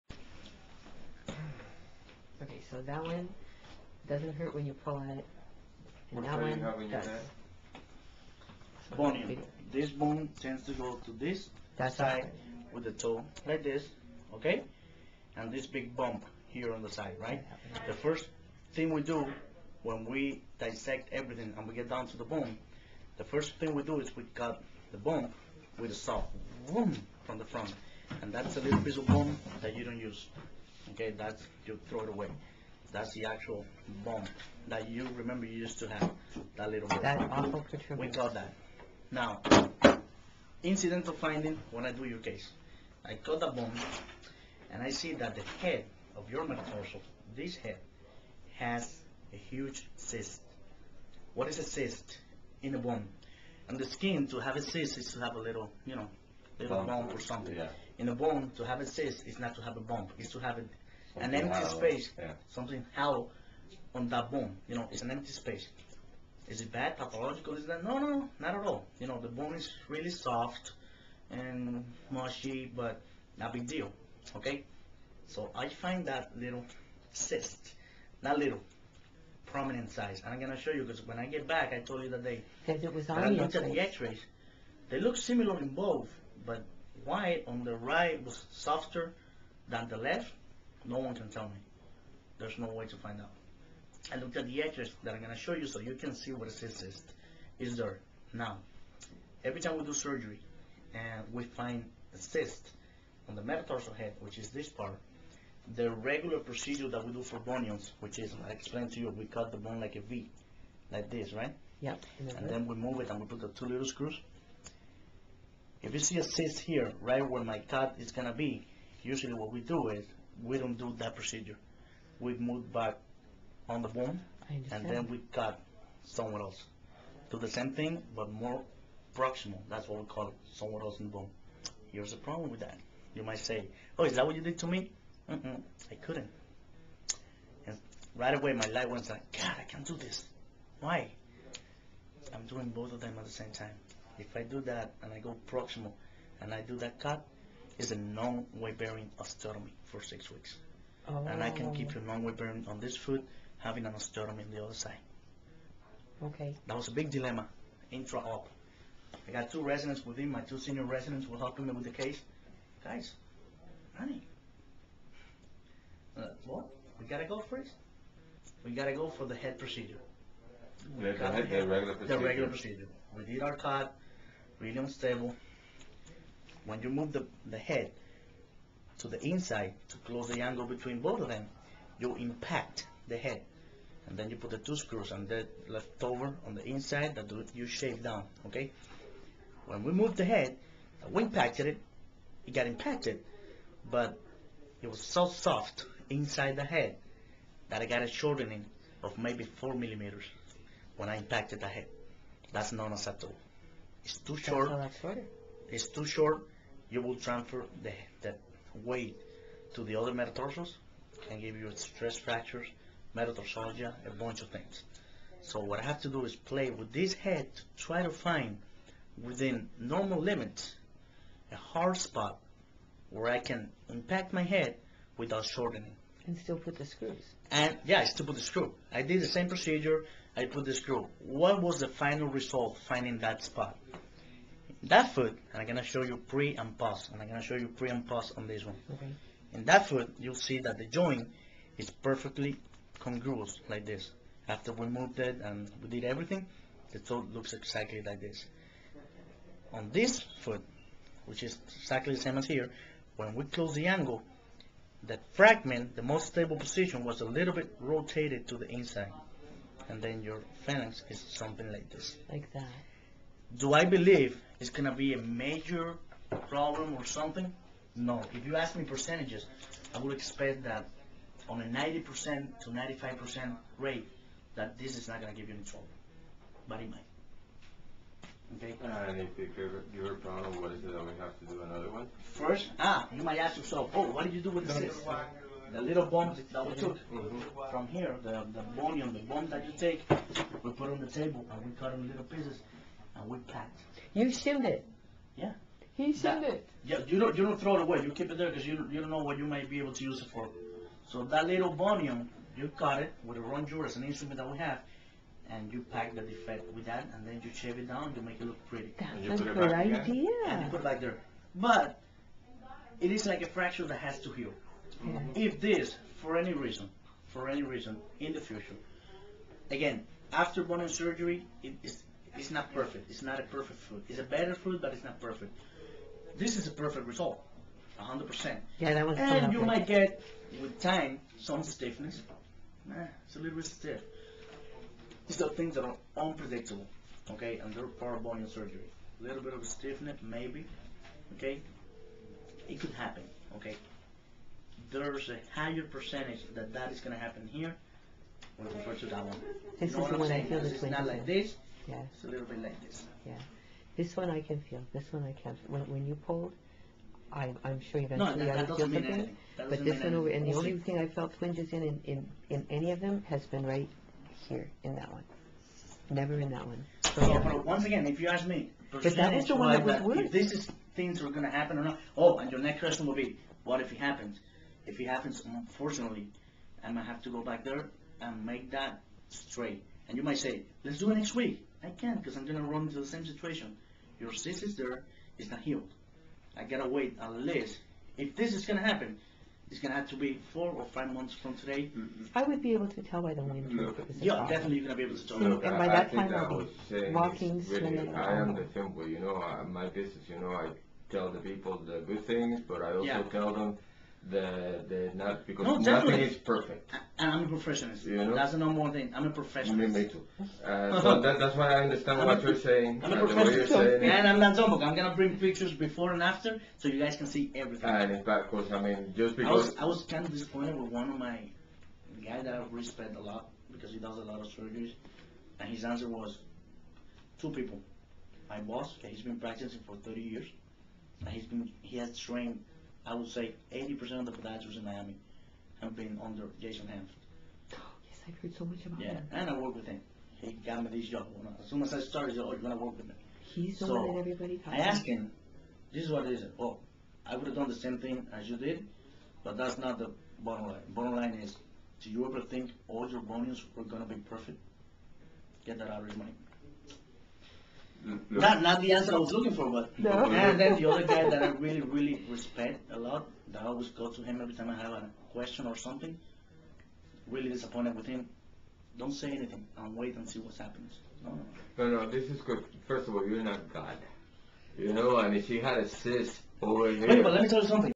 Bilateral bunionectomy 1st post-op appt 1 wk after surgery
This is Dr. Manuel Rodriguez of the South Miami Foot and Ankle Institute in Miami, Florida. He performed bilateral bunionectomy surgery on my feet June 5th. This is the appointment to remove the little casts on the feet, remove the staples from the incisions, see how they look and review what I should be doing for the next week. So far - all I've done for 7 days is lay in bed with my feet up and iced. No walking at all - used an adjustable commode next to the rented hospital bed for the first 5 days. As of Day 5, we worked out using an office chair to haul me to the bathroom and I lifted myself from chair to toilet and back. Got a wheelchair on Day 6 and spent a few hours tooling around but he would prefer I stay with the feet elevated